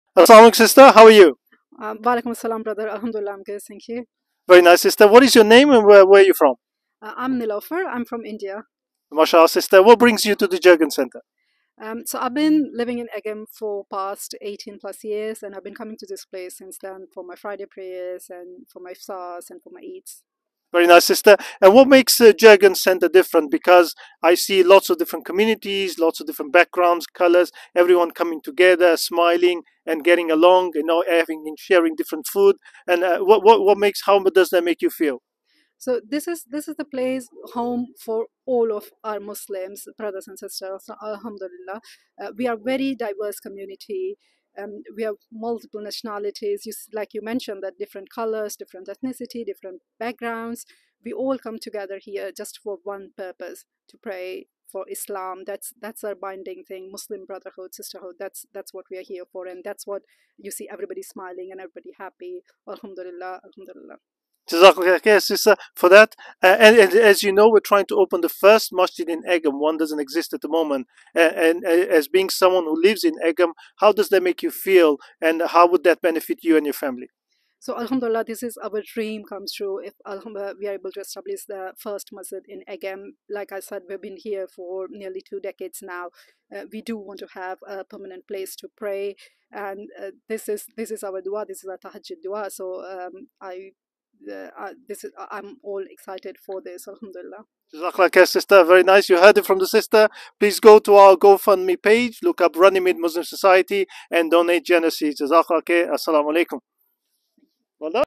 How would a local Masjid benefit you and your family? Interview with local sister from Egham
Runnymede Muslim Society